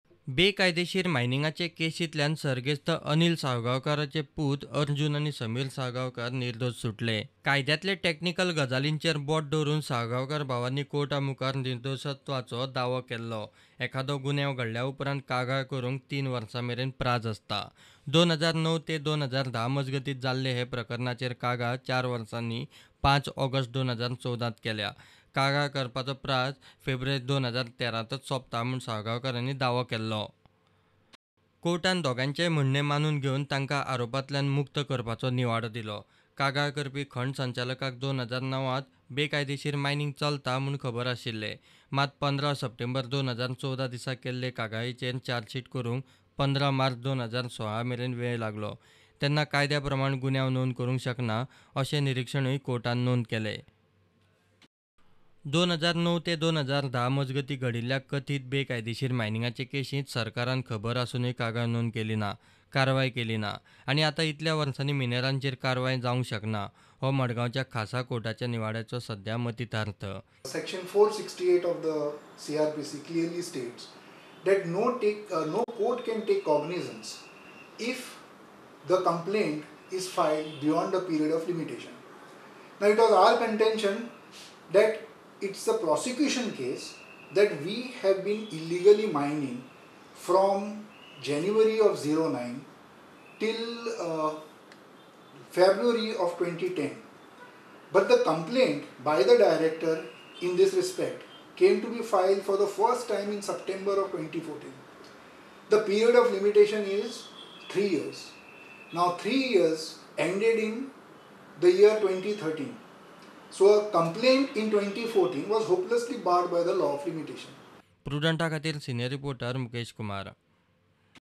ARJUN AND SAMEER SALGAOCAR DISCHARGED IN ILLEGAL MINING CASE_Prudent Media Goa
Margao:The Margao Special Court has discharged Miner Arjun Salgaoncar and Sameer Salgaoncar in illegal mining case at the stage of framing of charge. SIT (Mines) had chargesheeted them in 2016.

SIT in the chargesheet filed in 2016 had contended that accused were the Directors of S. Kantilal & Company Pvt. Ltd., during the period from January 2009 to February 2010.

Then the Company carried out mining activities at T.C. No.60/1952, situated at Asni Dongor in the villages of Curdi and Curpem of Sanguem Taluka, without grant of lease from the Directorate of Mines & Geology.

While the arguments before charge,  the defence side contended that complaint in the case dated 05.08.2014 is barred by limitation of three years as per law and therefore, the limitation expired in February 2013.

The Special Court consenting with the defence, observed that "if at all the accused were liable for punishment, itwould be as prescribed prior to the amendment of the year 2015, which is two years.Consequently, the period for taking cognizance of the offence by the Court would be three years."

It also observed that "Director of Mines, who is the complainant in this case, was aware of the alleged illegal mining as on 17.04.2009.But, the chargesheet was filed on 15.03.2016, on the basis of a complaint from the Director of Mines and Geology dated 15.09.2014. Hence, No cognizance of the offence can be taken as per law."